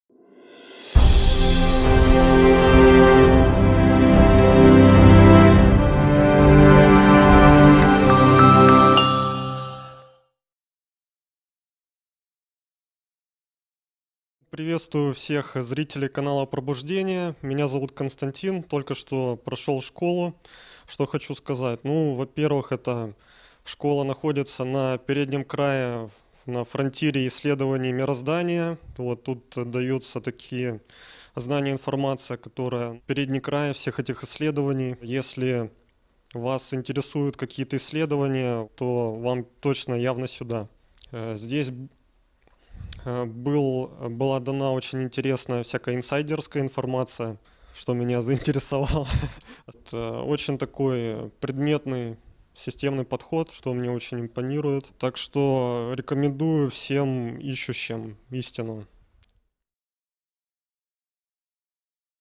Школа Регрессологов Святослава Игнатьева. Москва, декабрь 2019. Отзыв Константина
Международный Исследовательский Центр "Пробуждение" представляет:
авторскую Школу регрессологов Святослава Игнатьева "Обретение целостности через связь времён" Подробнее о Школе и условиях обучения

В декабре 2019 года в Москве состоялся очередной курс Школы. Предлагаем Вам отзывы выпускников, многие из которых уже успешно практикуют.